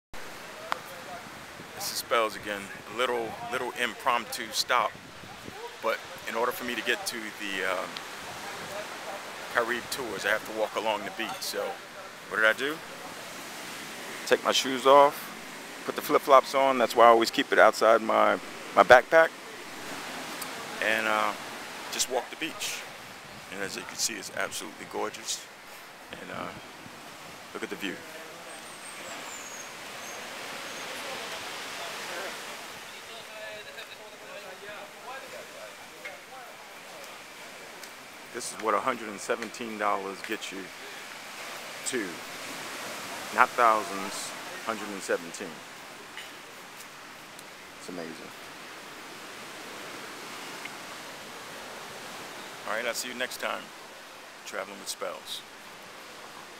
Travelwitspells Dominican Republic
travelwitspells Traveling from Sosua to Santiago Dominican Republic by bus but I am walking the beach to Caribe Bus lines to save money..Lol